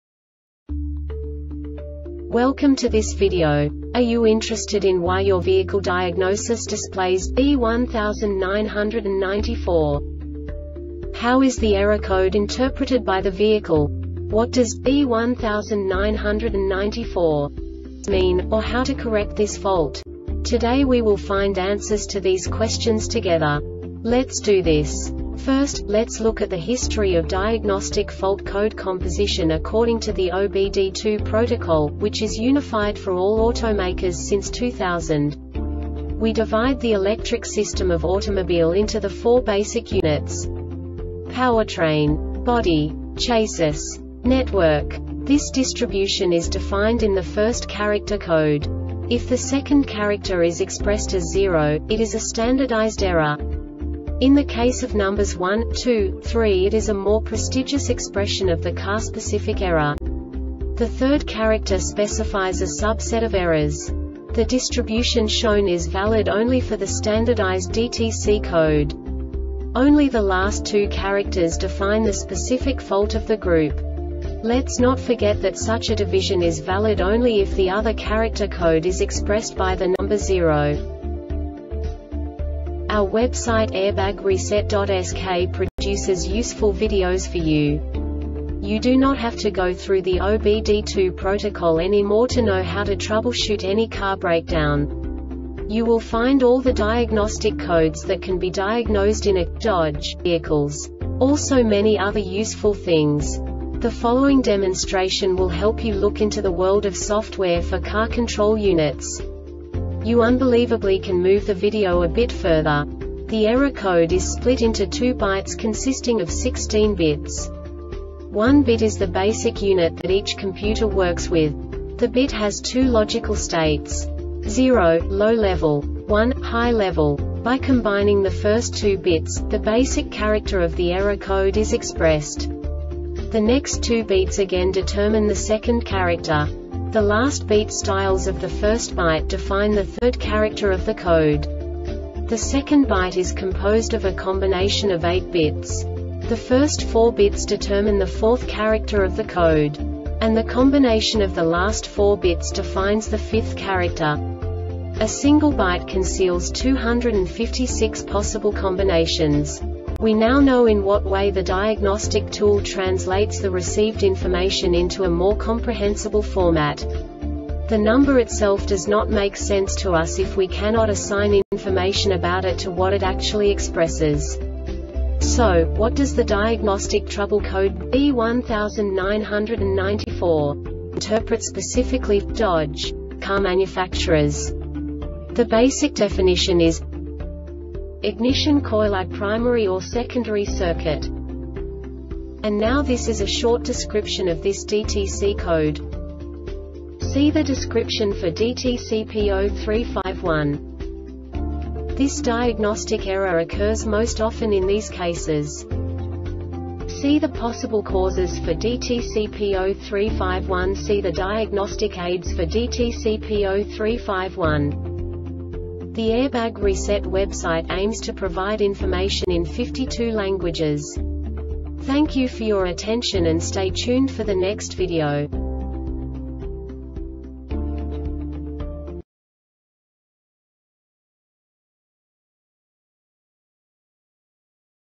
DTC Dodge B1994 Short Explanation
Contents:
0:21 Basic DTC analysis according to OBD2 protocol standard.
1:48 Insight into programming
2:58 A diagnostically understandable description of the Diagnostic Trouble Code
3:05 Basic error definition

Make:
Dodge
Code:
B1994
Definition:
Ignition Coil I Primary or Secondary Circuit
Description:
See the description for DTC P0351.
Cause:
See the possible causes for DTC P0351.
Diagnosis:
See the diagnostic aids for DTC P0351.